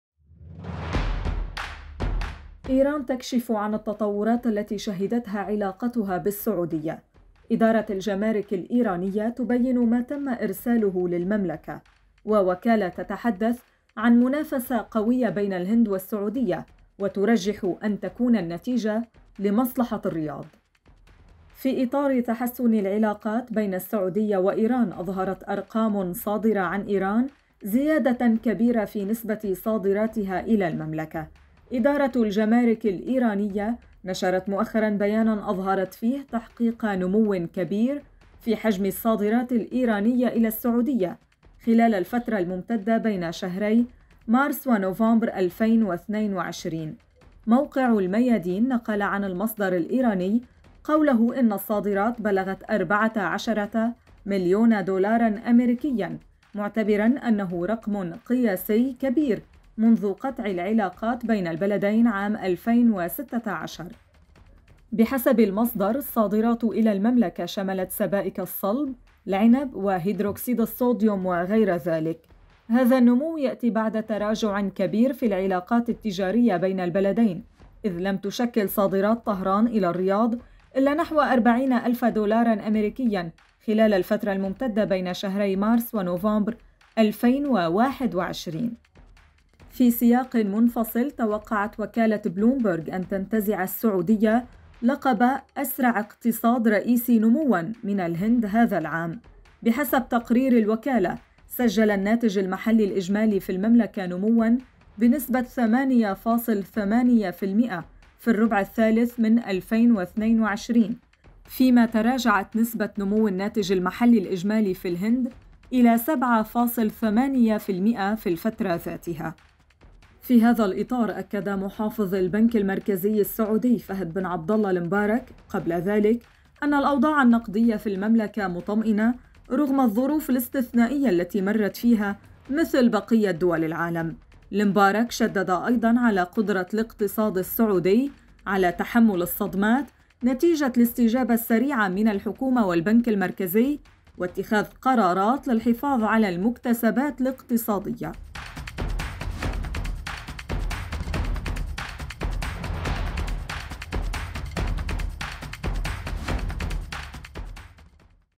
ايران تكـشف عمّا ارسلته لـ السعودية بعد تحسن العلاقات والمملكة إلى المقدمة ! Saudi arabia
في إطار تحسن العلاقات بين السعودية وإيران، أظهرت أرقام صادرة عن إيران زيادة كبيرة في نسبة صادرتها إلى المملكة. إدارة الجمارك الإيرانية نشرت مؤخرا بيانا أظهرت فيه تحقيق نمو كبير في حجم الصادرات الإيرانية إلى السعودية خلال الفترة الممتدة بين شهري مارس ونوفمبر 2022